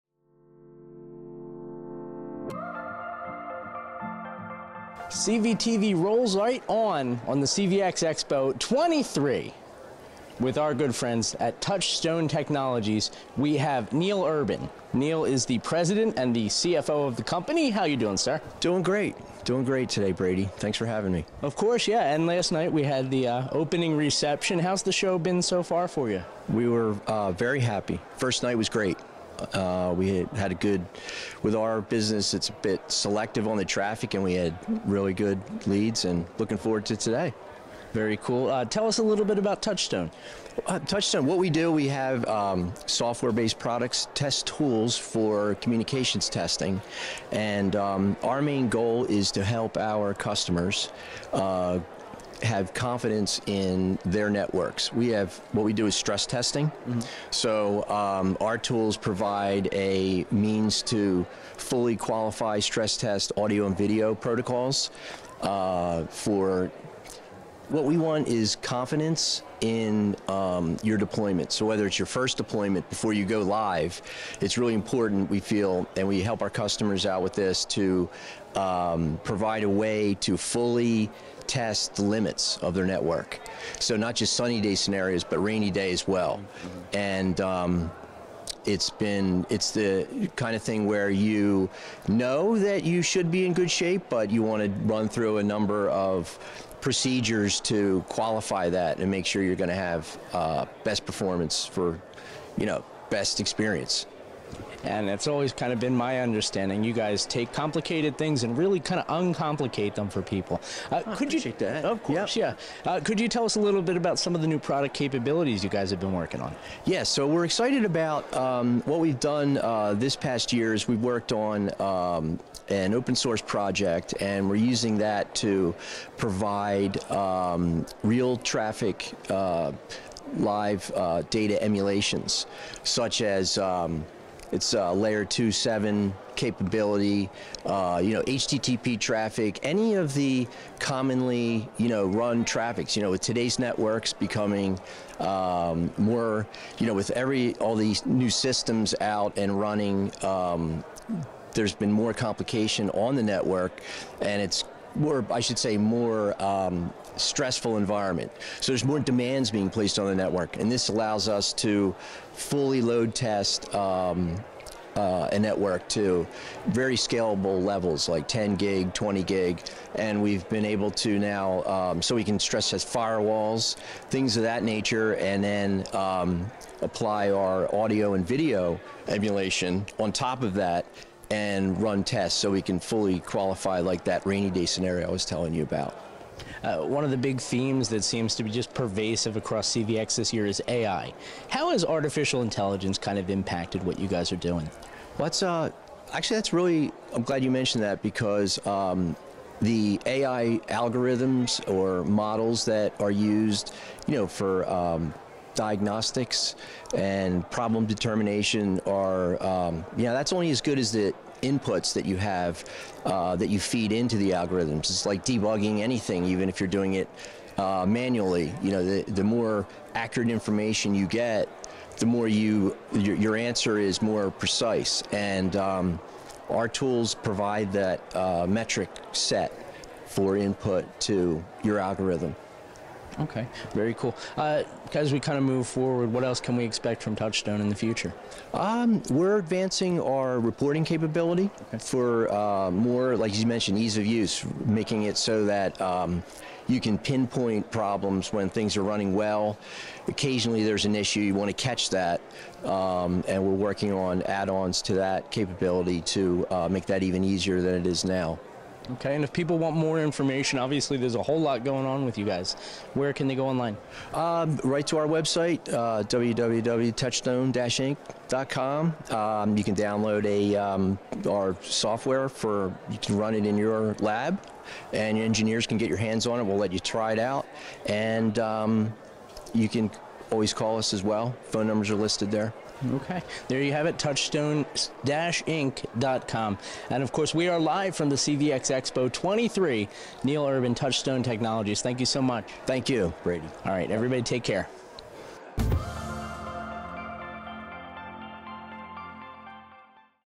Touchstone Technologies Discusses New Testing Product Capabilities at CVx
CV TV welcomes Touchstone Technologies President and CFO, Neil Urban. In this interview, he discusses the company’s recent product and service releases, as well as new product capabilities such as network traffic stress testing, application traffic replication and AI. This interview was recorded from the showroom floor of the CVxEXPO23 at the Talking Stick Resort in Scottsdale, A.Z..